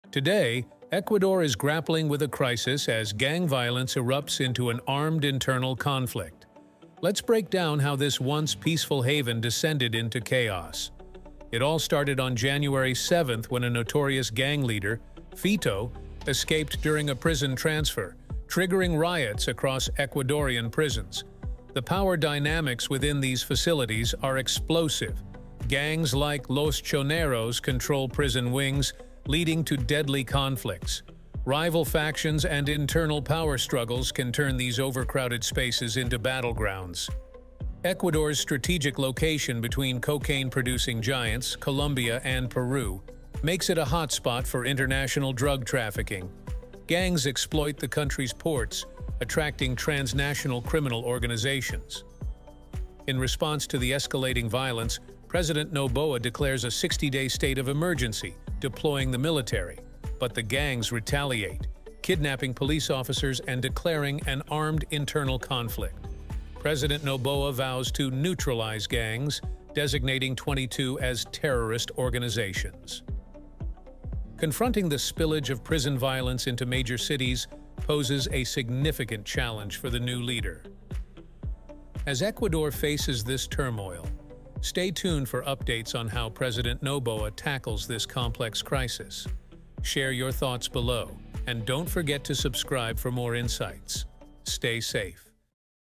Ecuador descended gang violence, President Noboa
Ecuador faces a crisis as gang violence escalates, prompting President Daniel Noboa to declare an "armed internal conflict" and a state of emergency.